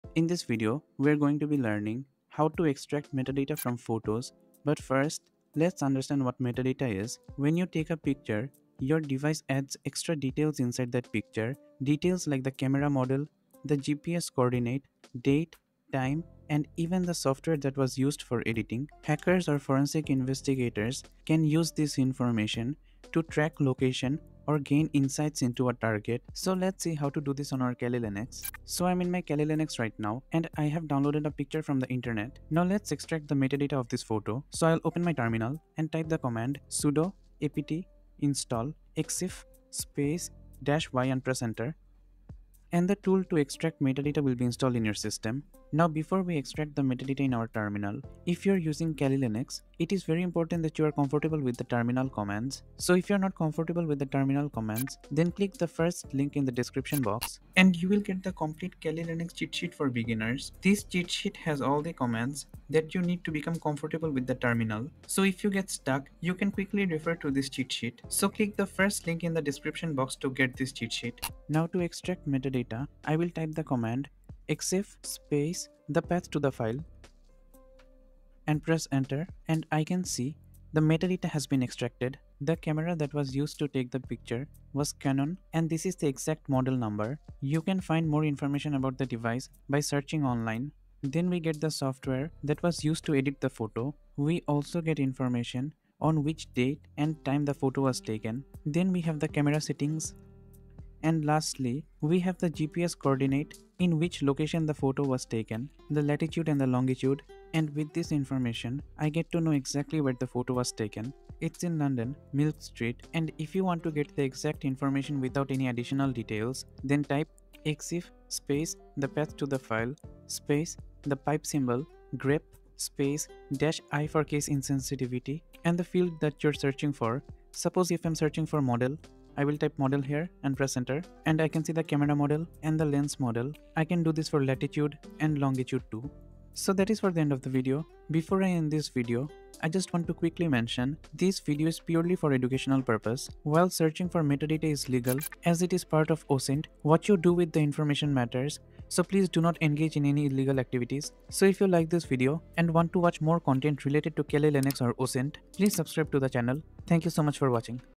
How Hackers Extract Metadata from Photos | Kali Linux
In this video, we will learn how to extract metadata/Exif data from photos using Kali Linux. Metadata can reveal hidden information such as camera details, timestamps, and GPS coordinates embedded within image files.